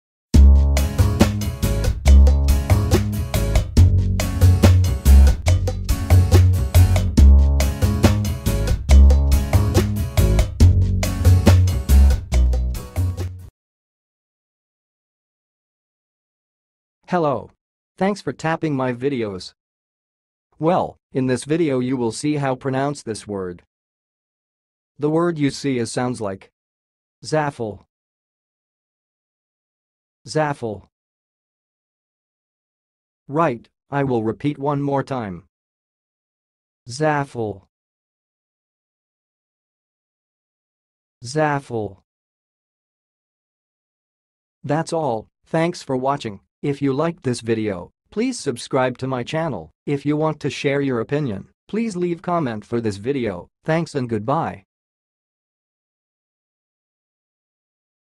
ZAFIL - HOW TO PRONOUNCE IT!?
Watch how to say and pronounce "zafil"!